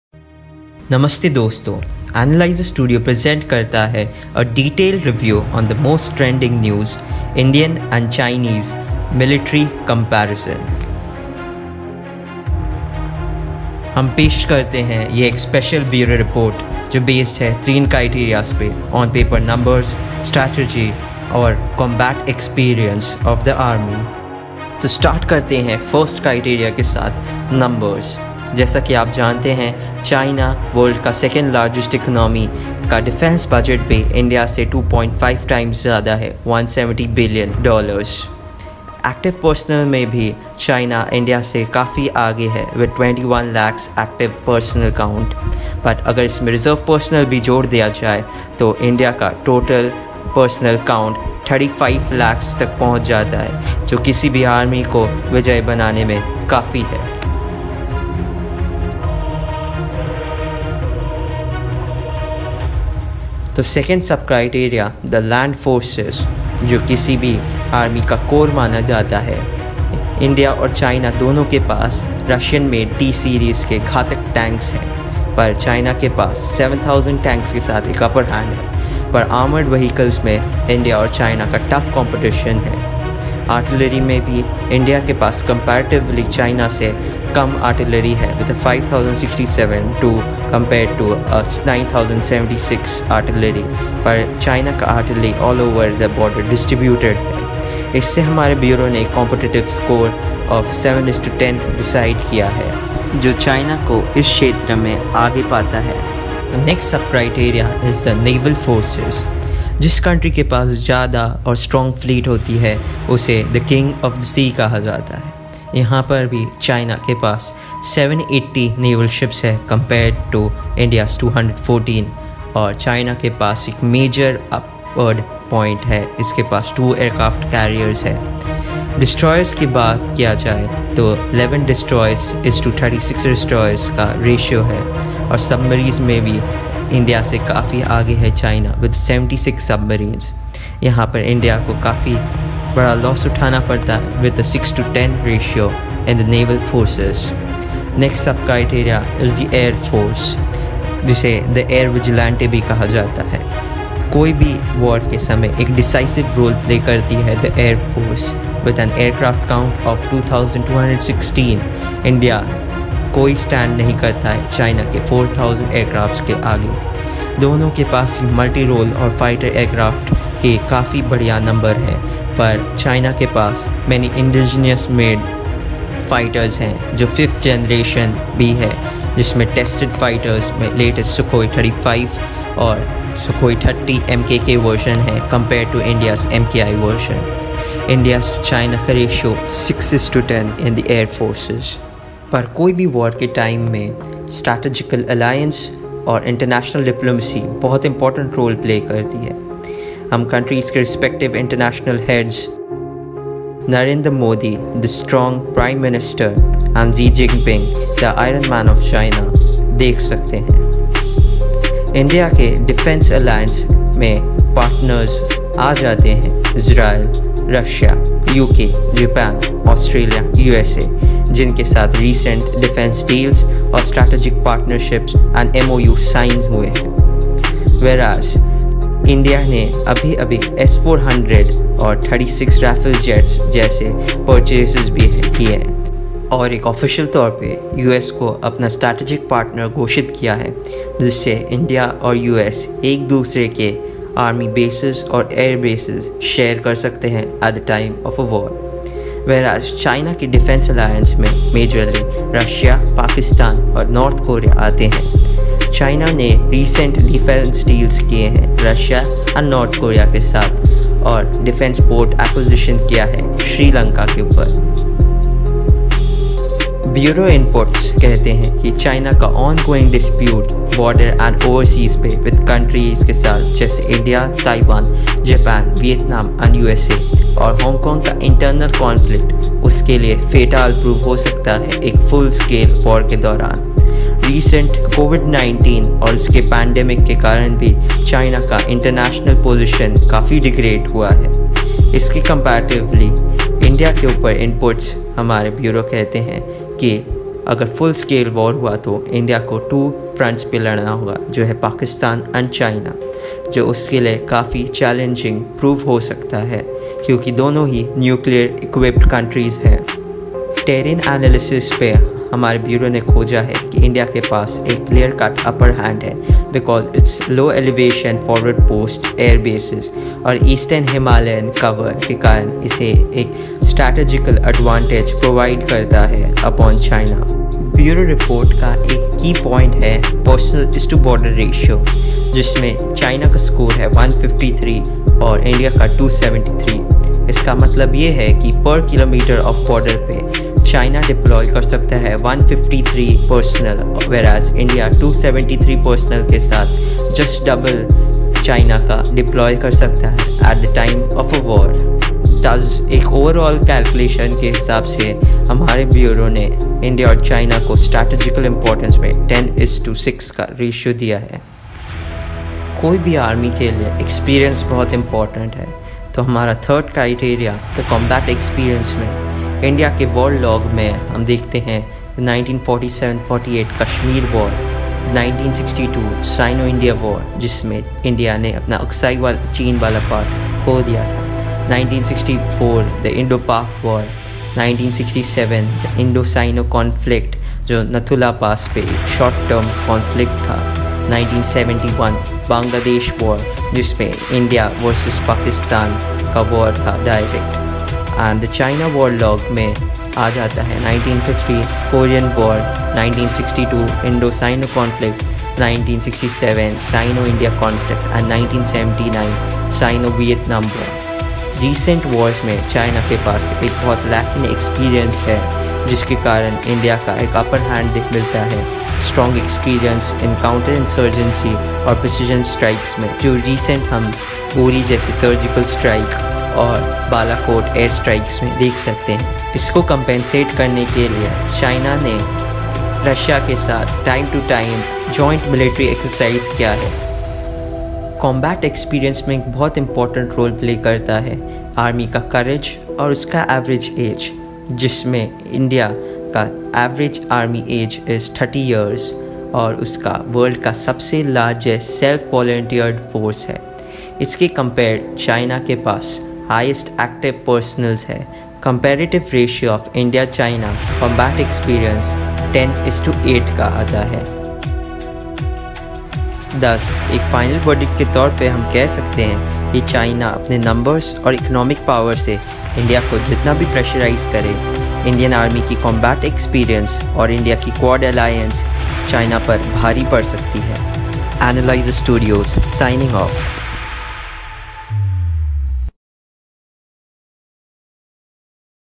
India vs China 2020 : Most Brutal Military Comparison| Infographics | Figures| Insider Review
This Video is the Most Analytical and Graphical  Comparison of two Nuclear Weapons armed Countries .Lets See In a Face Off  who will Win ???